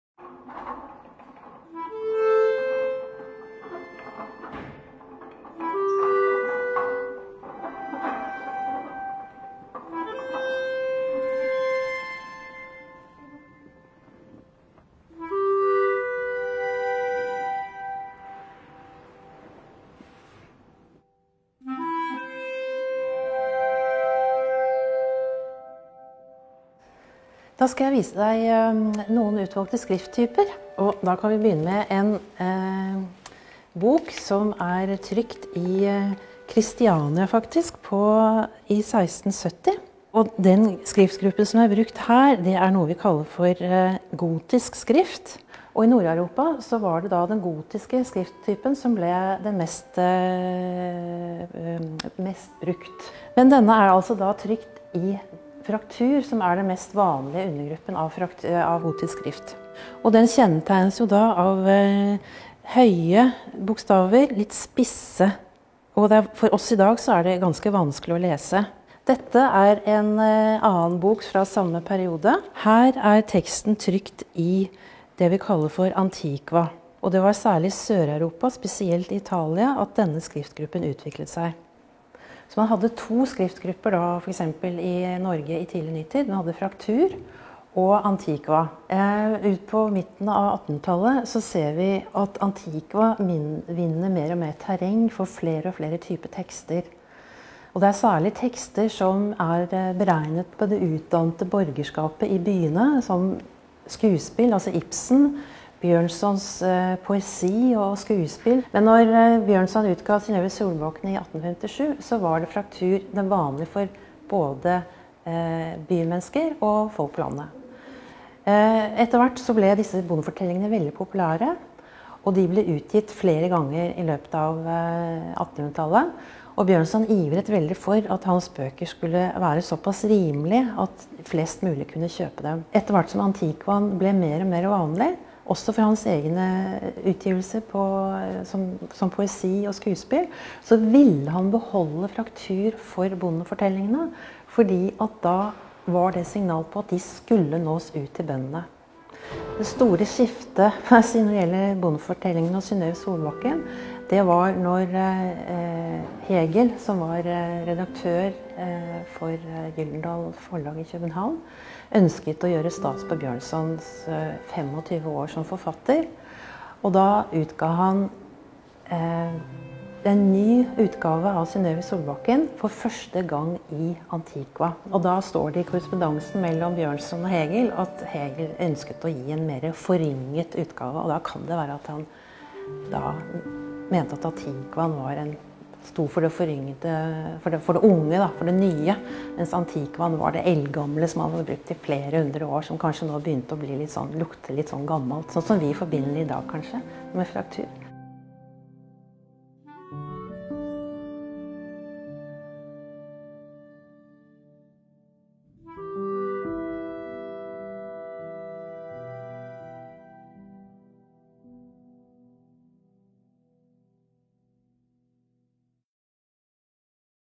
Bjørnson til bøndene
Visste du at Bjørnstjerne Bjørnson ville at historiene om Synnøve Solbakken skulle trykkast på ein måte som gjorde at dei nådde bøndene spesielt?

På 1800-talet hadde vi to skrifttypar i Noreg. Den eine blei i aukande grad brukt til høg-kulturelle tekstar som romanar, dikt og skodespel retta mot byborgarskapet. Men bøndene las framleis tekstar som hadde den andre skrifttypen, som skillingsviser, forteljingar og annan populærlitteratur. Dette var Bjørnson oppteken av.

Vil du lære enda meir om den norske bokhistoria, kan du besøke utstillinga vår som feirar at den norske trykte boka er 500 år. Utstillinga er gratis og står fram til 18. januar 2020: